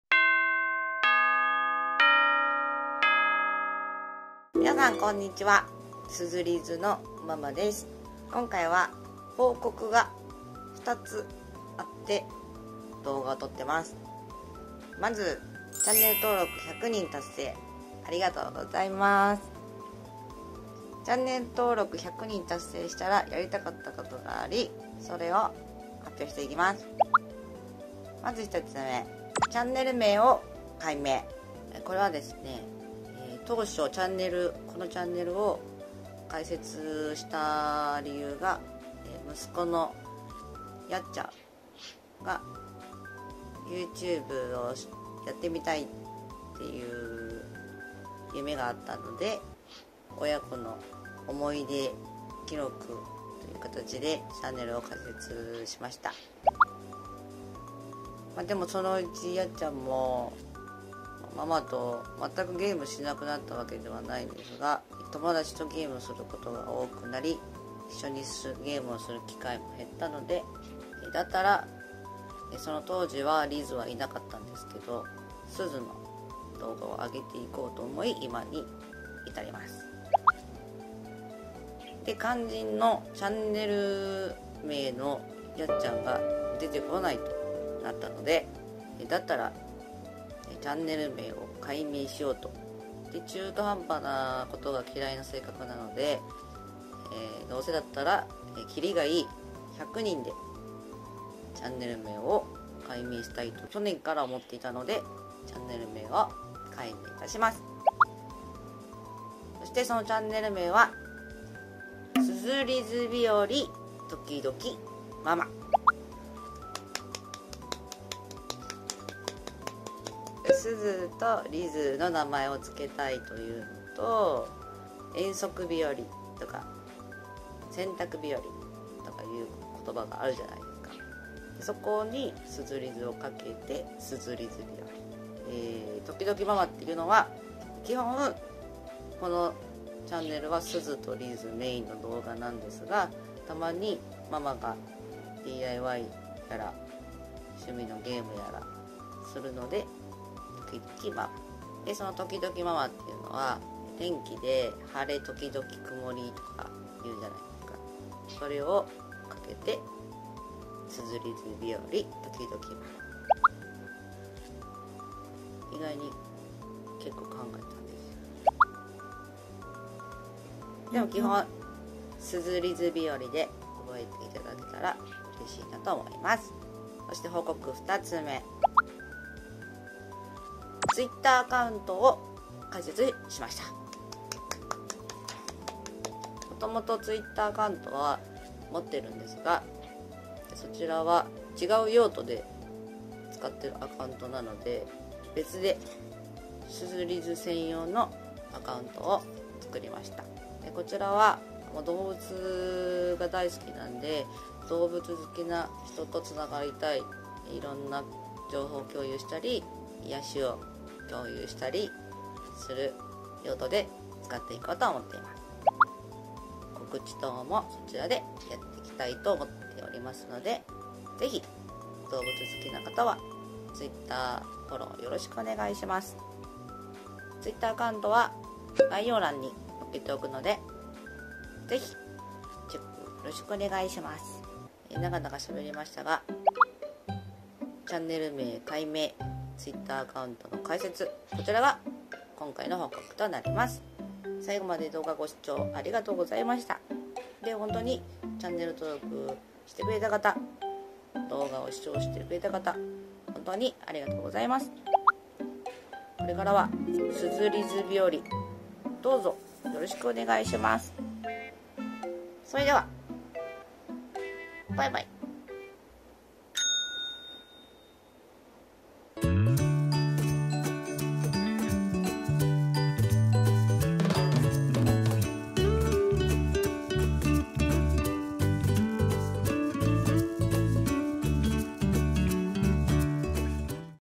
【ご報告】いつも本当にありがとうございます♡
沢山の方にすずりずを見てもらえて嬉しいです♡
今回はチャンネル名改名とTwitter開設のご報告です！
これからも親バカ全開ですずりず動画を出していきたいと思っていますのでよろしくお願いします😊